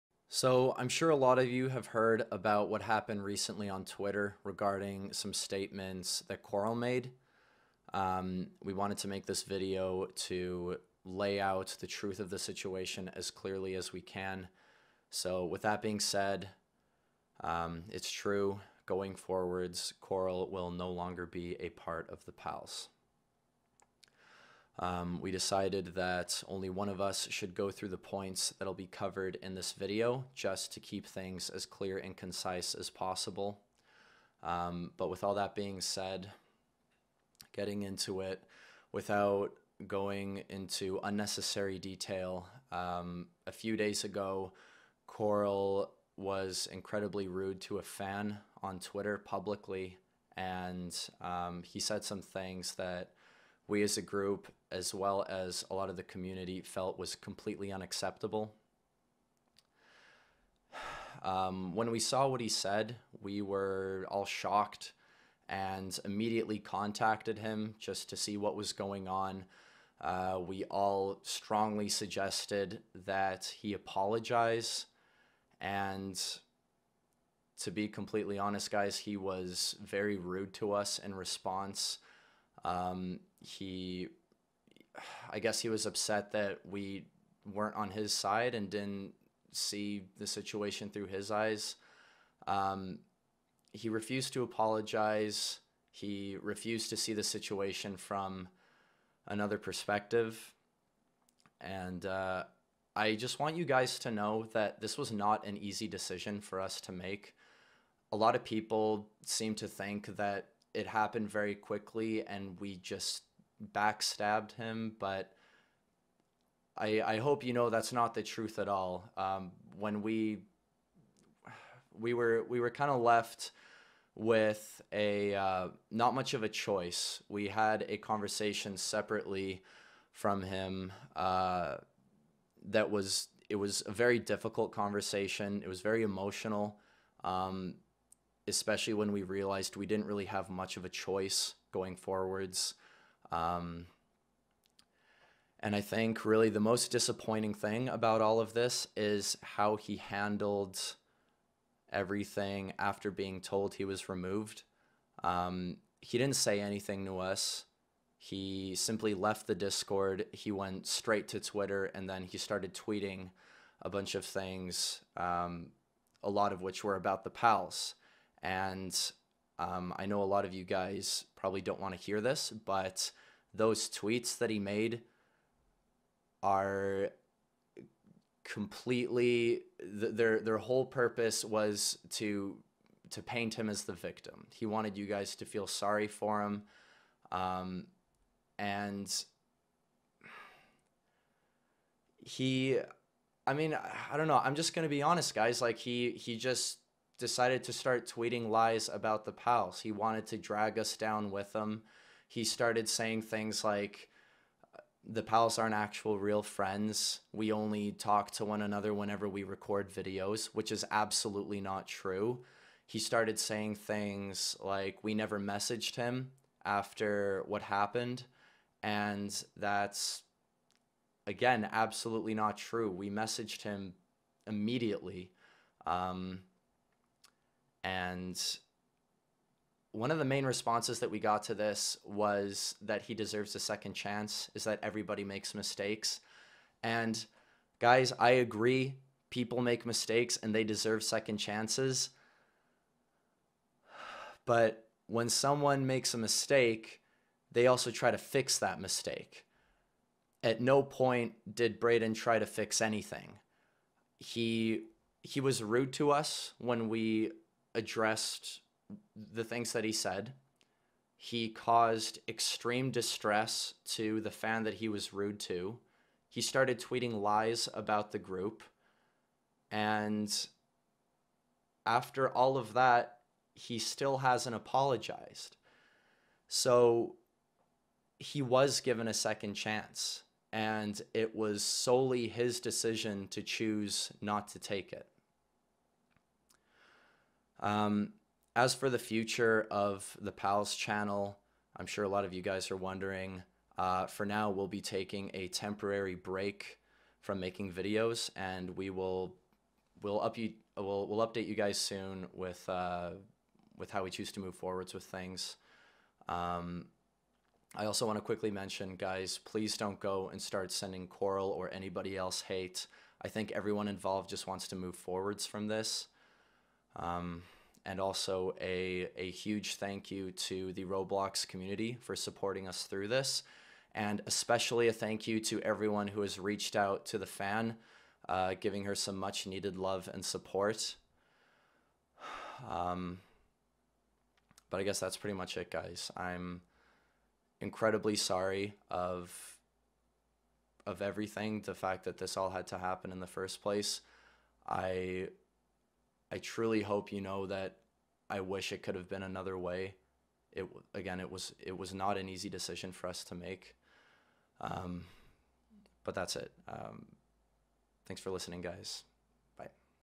Why we removed Corl from The Pals [Reuploaded]
This video was made private on The Pals channel.